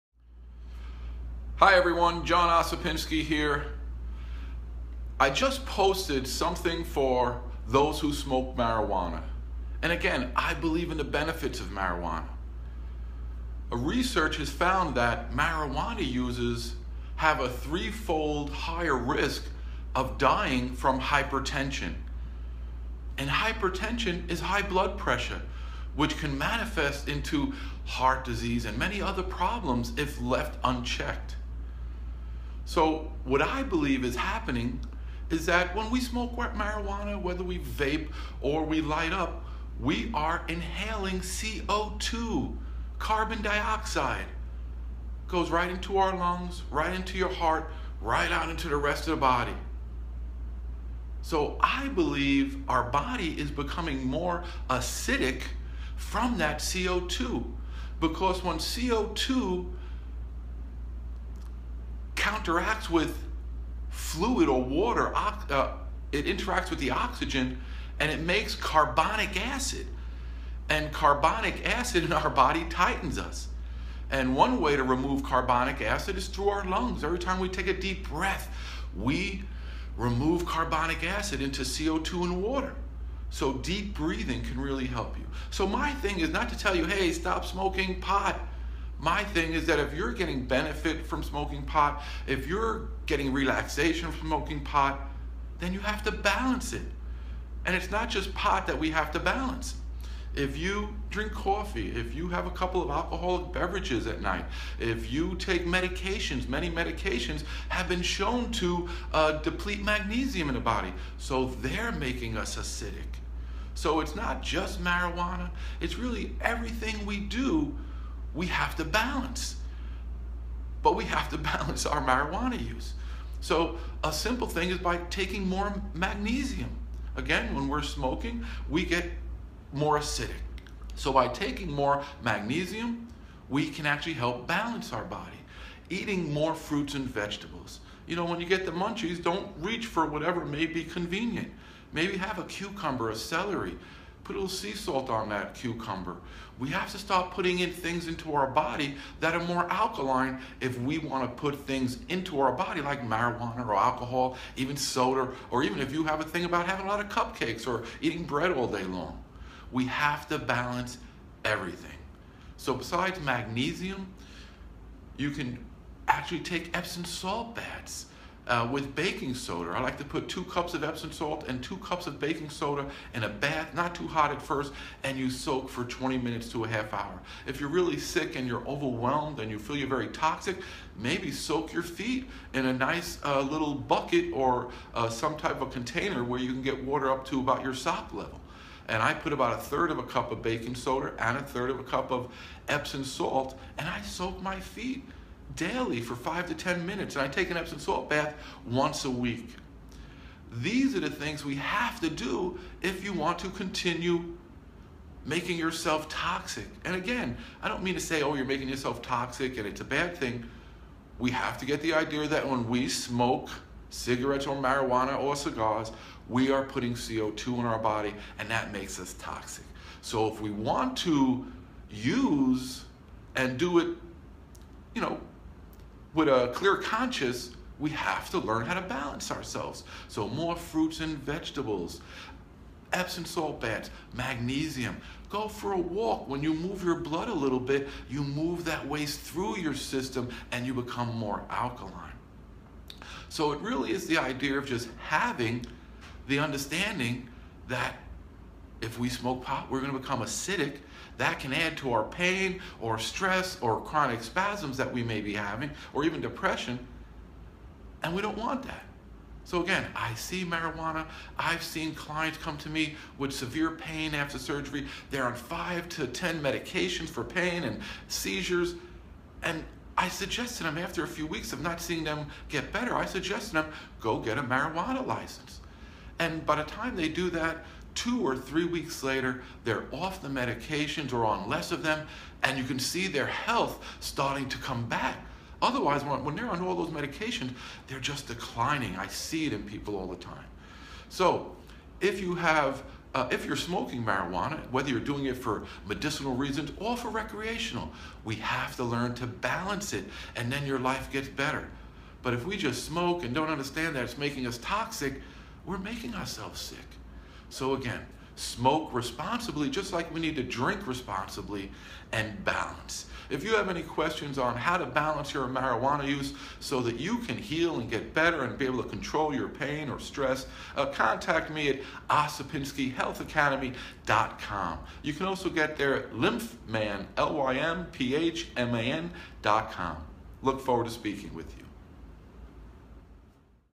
Balance Marijuana Use to Help Prevent Hypertension
If you use marijuana for recreational or medicinal purposes you need to balance marijuana use to help prevent hypertension, high blood pressure, and toxicity. When you smoke, you inhale CO2 (carbon dioxide) which can interact with the water in your lungs to produce carbonic acid. To smoke marijuana responsibly you need to reduce the toxicity that is produced by smoking. Learn to improve your lymphatic system to use marijuana responsibly.